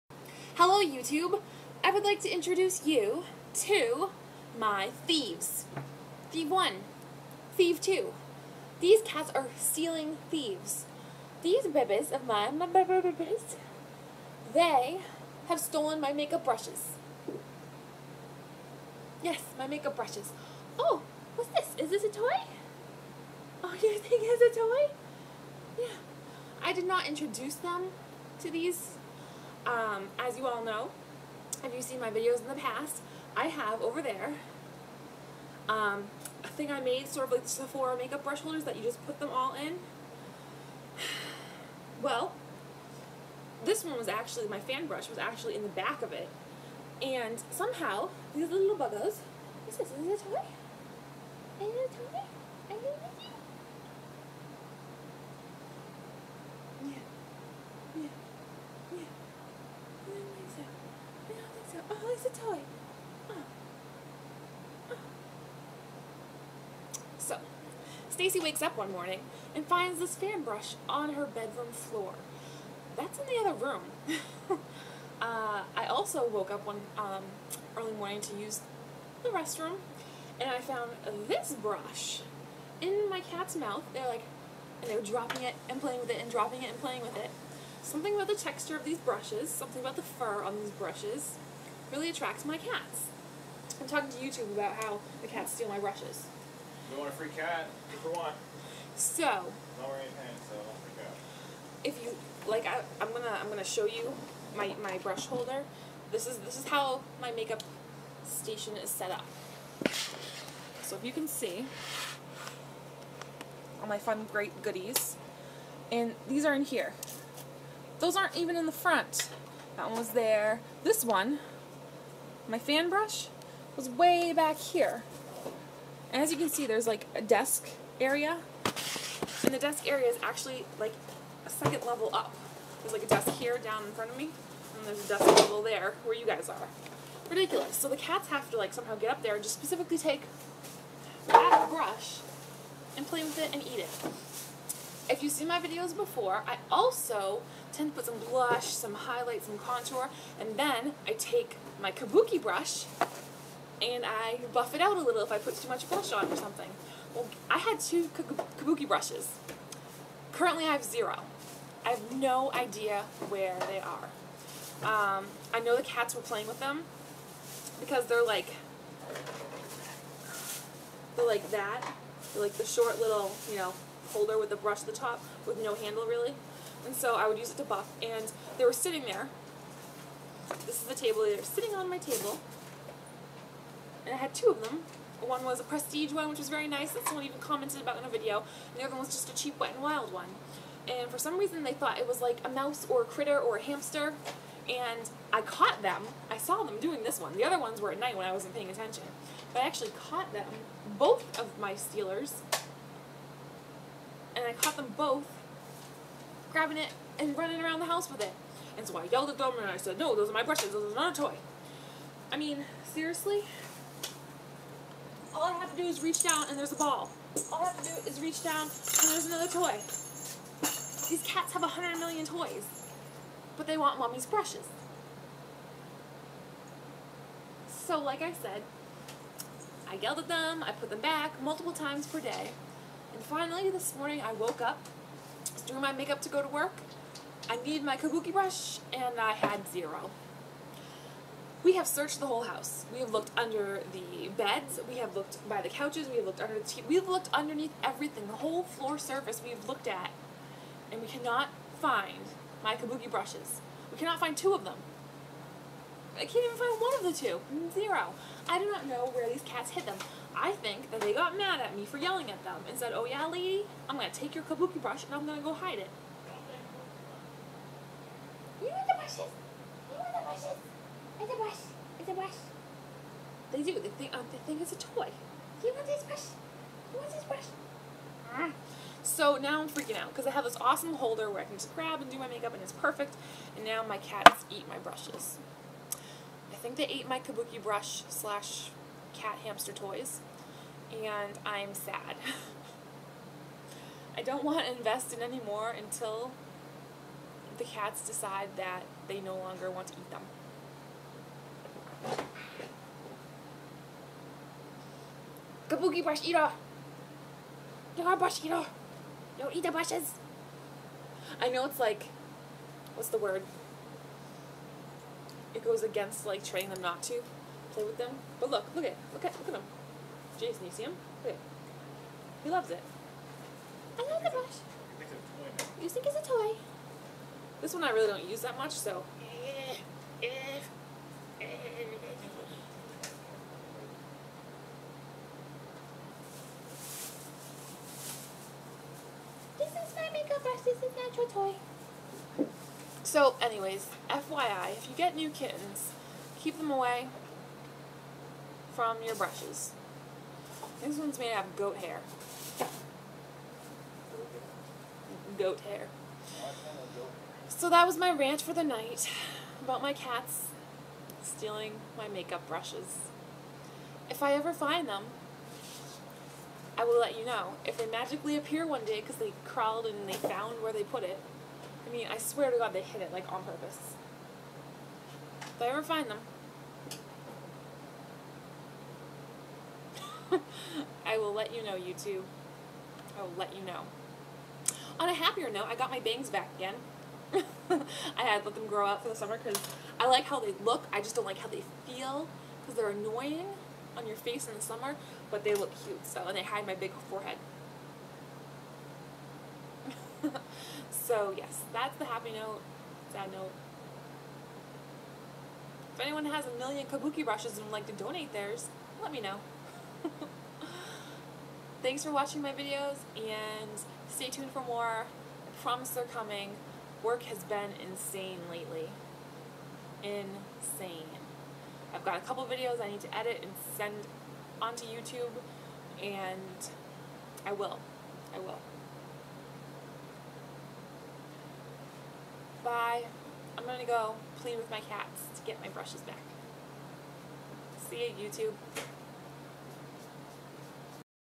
Kitten Thieves: Kabuki Brush Edition
Duncan and Oliver have stolen my Kabuki Brushes...I have also found them with my fan brush and angled brushes...argh.  I can't even find one of the 2 kabki brushes!  :(  Such a loss.  It was cute when I caught them carrying the brushes around in their mouths like they were hamsters or something, lol.